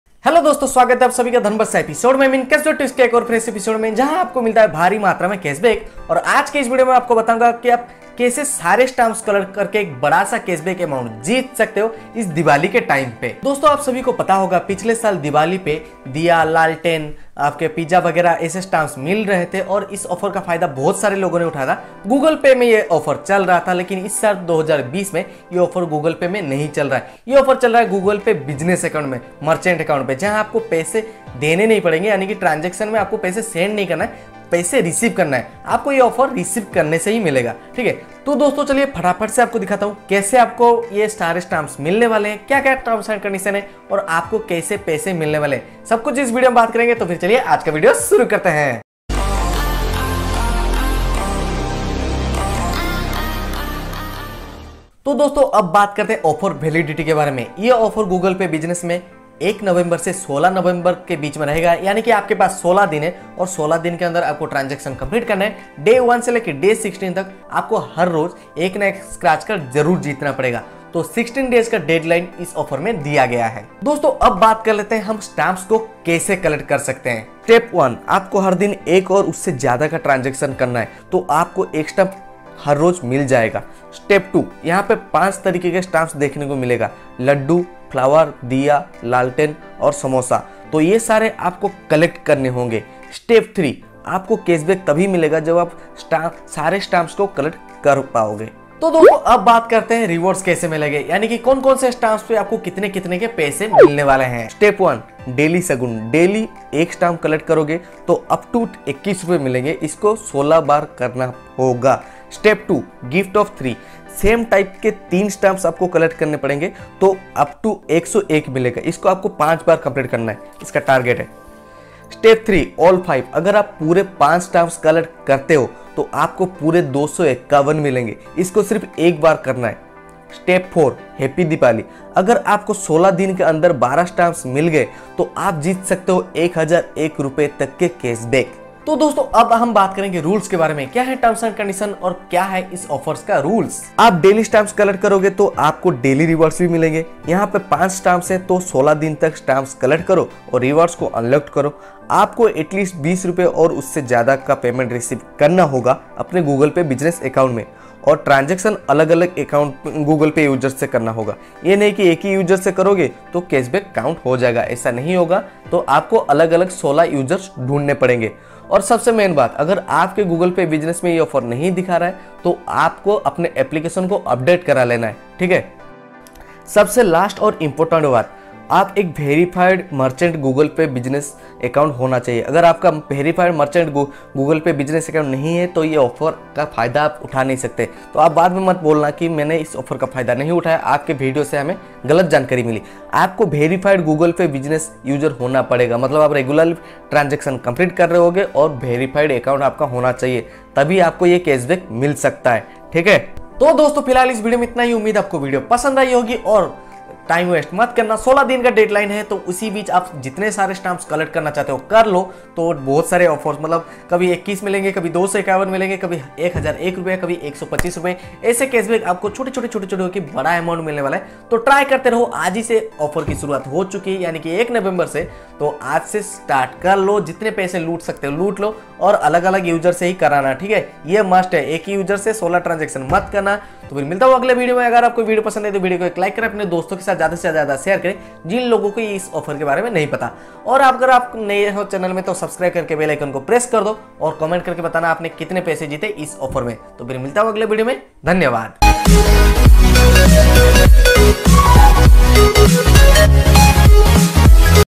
Google pay Business offer// Har din diwali & collect stamps // Big rewards 🔥🔥🔥
In this video you know about the latest Google pay Business offer on diwali. Collect all stamps and win big rewards.

      I will Definitely Give you credit in my Description Box. thank You..